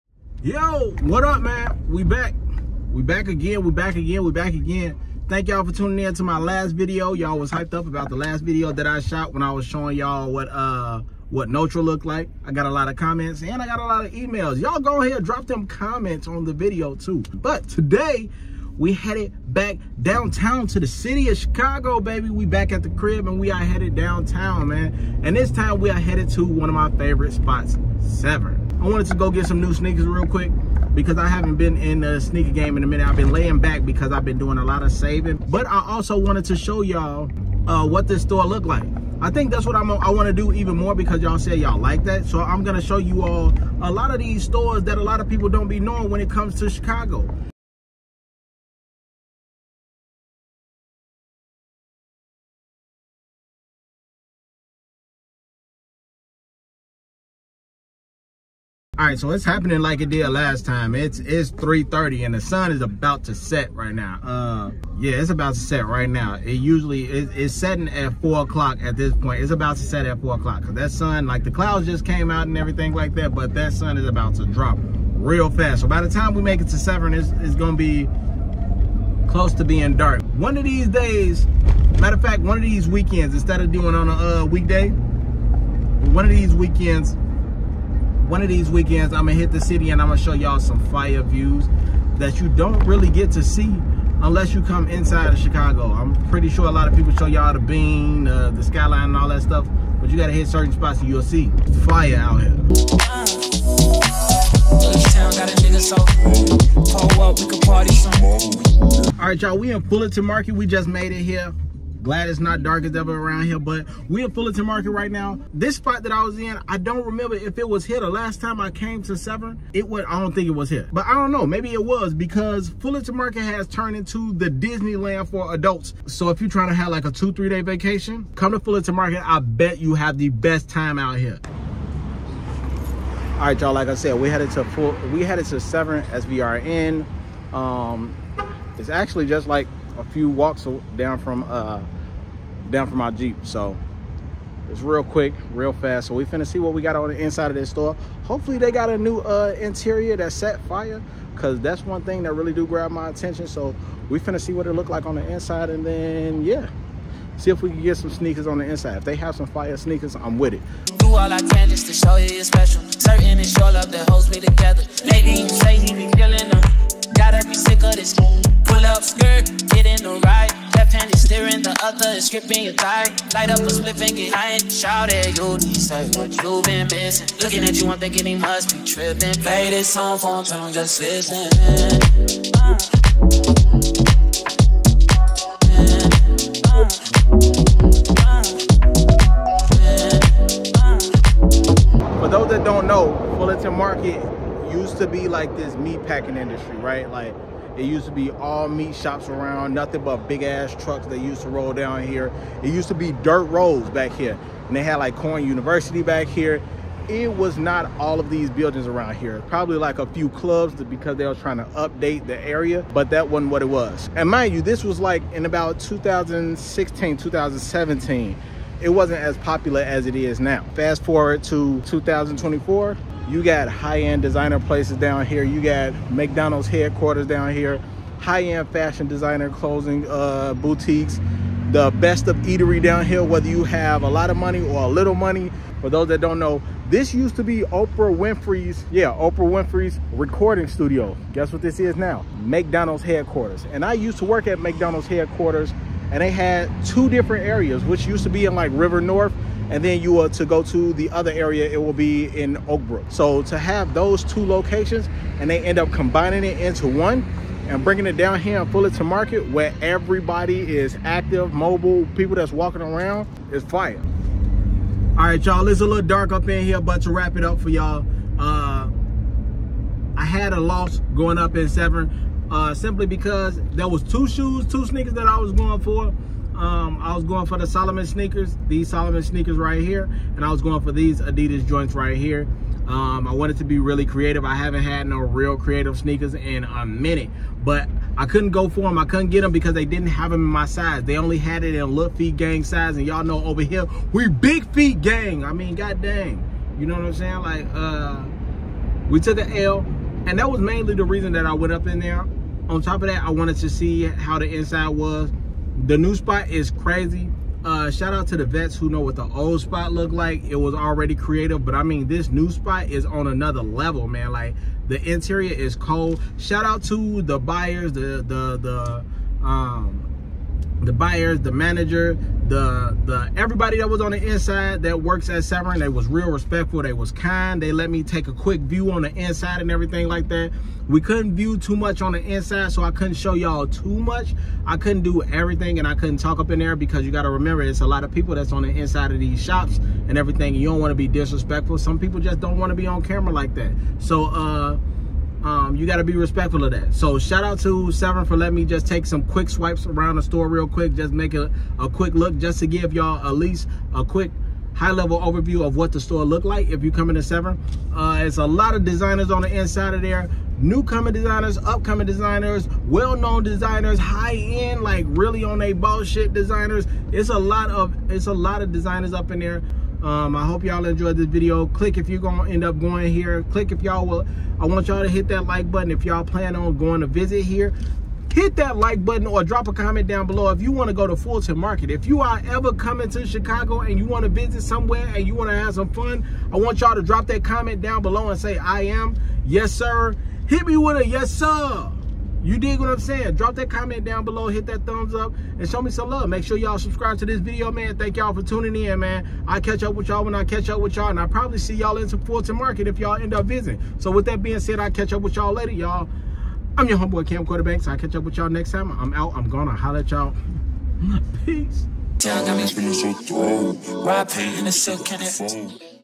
Shopping In Chicago | SVRN | High End Fashion Boutique, Sneakers and More!
We back in the city!! But this time we’re headed to the High-End Fashion Boutique in Fullerton Market Chicago. We’re going to SVRN! From fashion-forward clothing pieces to creative sneakers: we’re about to take a peak inside this styling shop. Maybe pick up a few scents, some sneakers, and maybe a jacket or two. Let’s see.